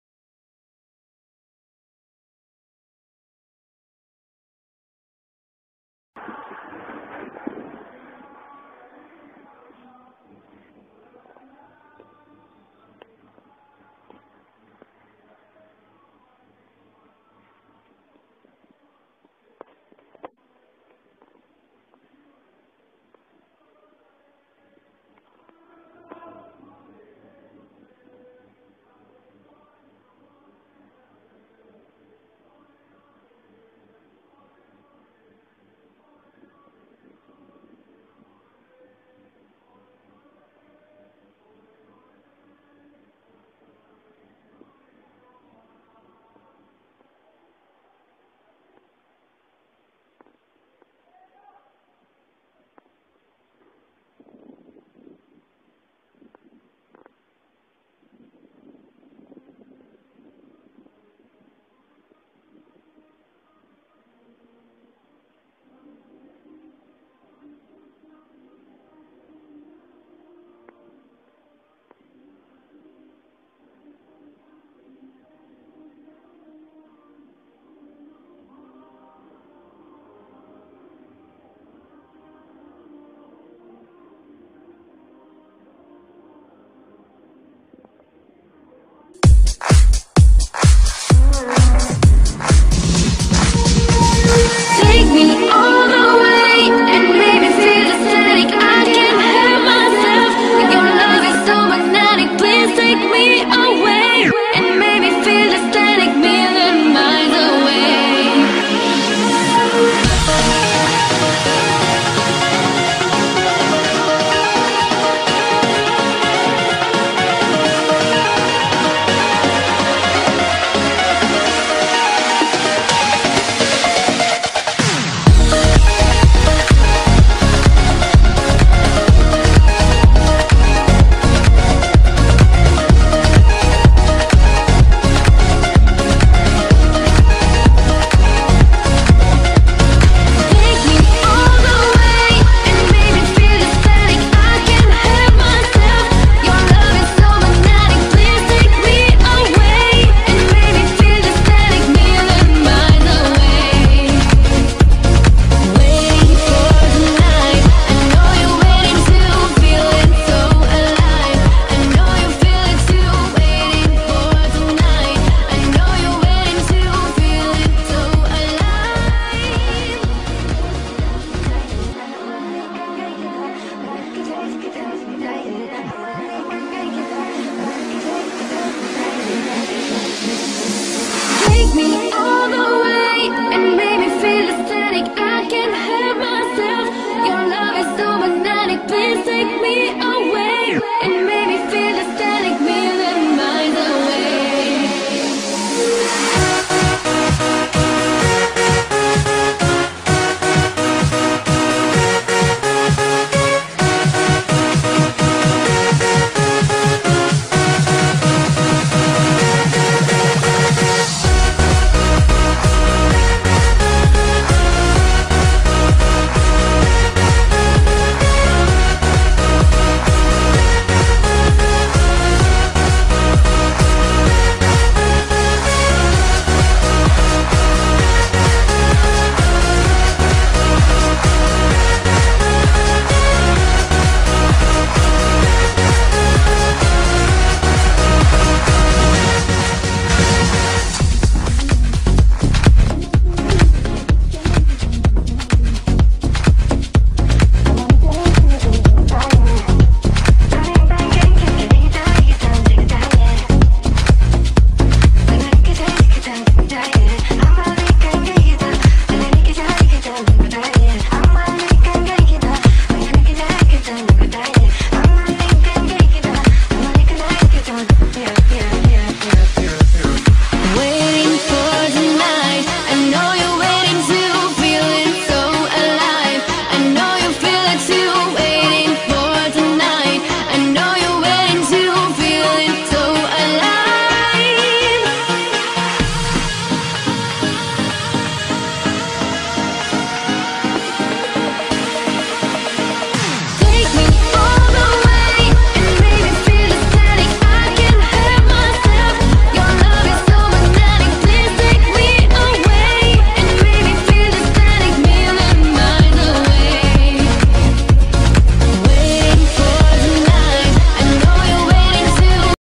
Charaf'Eddine El Ouardi, Match Demi-Final Councours Gillette Lbess L'10 "2014" - Ch #1
Charaf'Eddine El Ouardi, Gardien de But Councours Gillette Lbess L'10 . Match Demi-Final à Casablanca, Morocco Avec Entraineur Aziz_Bouderbala est Entraineur des Gardien de But
Ch #1

Facebook Pages : Charaf Eddine El Ouardi